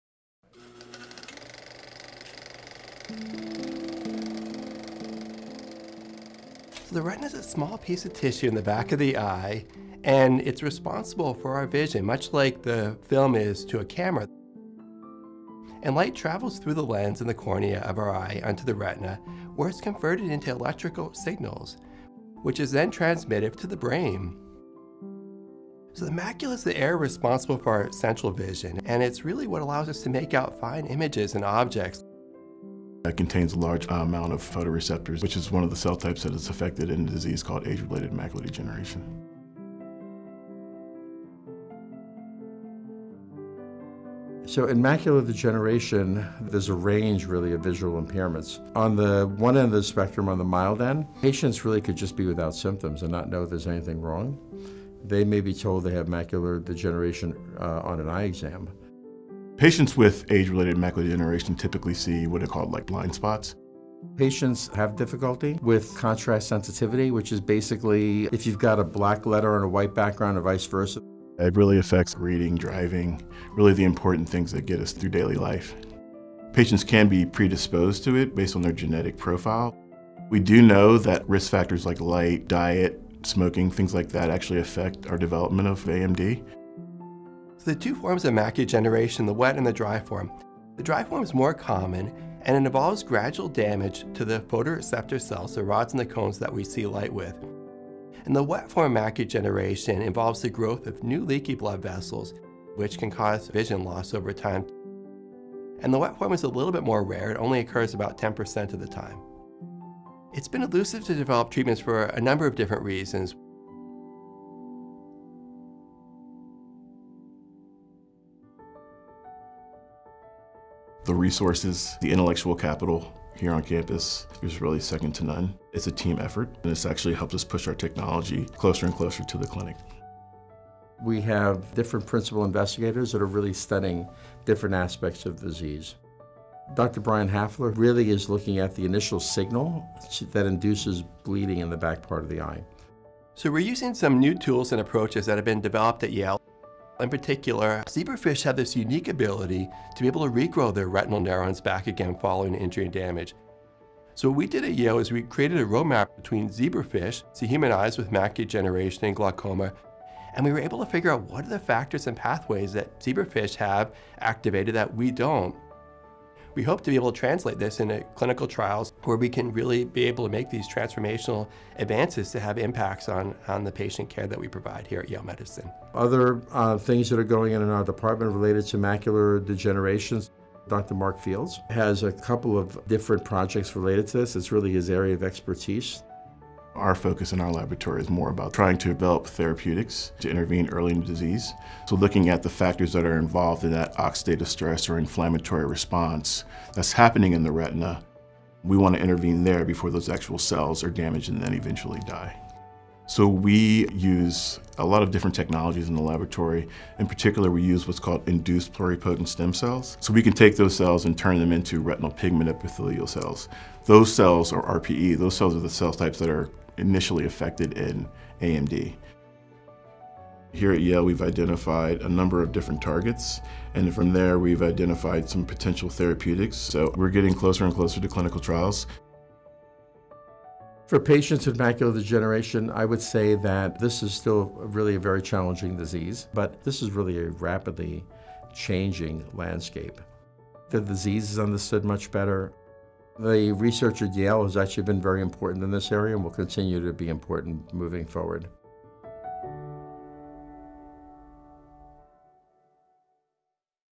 Macular Degeneration - Yale Medicine Explains
As we age, it's important to get regular eye exams to check for a condition called macular degeneration. It's the leading cause of loss of visual acuity or sharpness and the most common cause of legal blindness for people over age 65 in the United States.  Macular degeneration can negatively impact your life by affecting your ability to drive, read, do computer work and even recognize faces. Yale Medicine eye specialists are at the forefront of developing better therapies for age-related macular degeneration. They have improved their success in treating it to as high as 90 percent with some retinal therapies.

0:00 The Eye
0:39 Macular Degeneration
1:54 Research at Yale